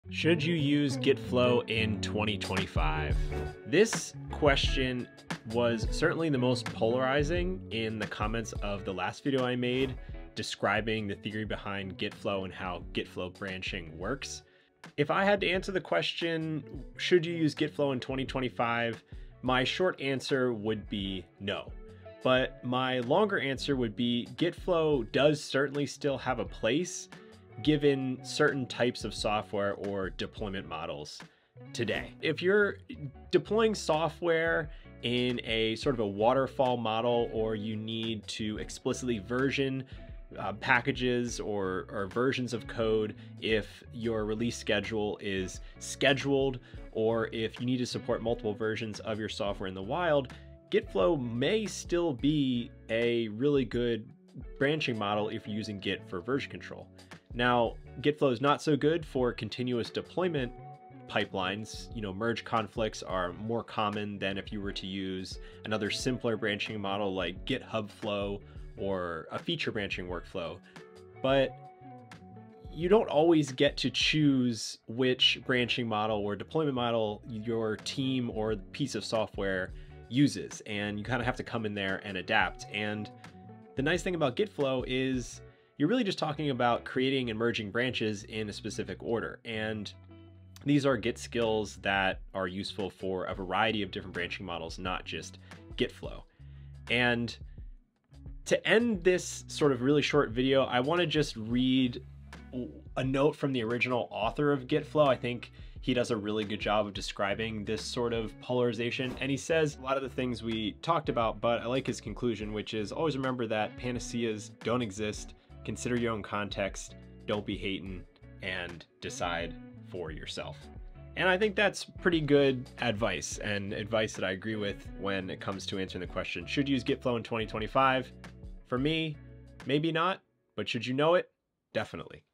Should you use Gitflow in 2025?
Gitflow was conceived by Vincent Driessen. Check out his original blog post here for an in-depth explanation, diagrams and code samples

Curious about Gitflow? You're not alone. If you support software that has a scheduled release structure, or operate under a waterfall development model Gitflow may be an interesting model to learn about; however, with the rise in popularity of CI/CD other (simpler) branching model may be more applicable.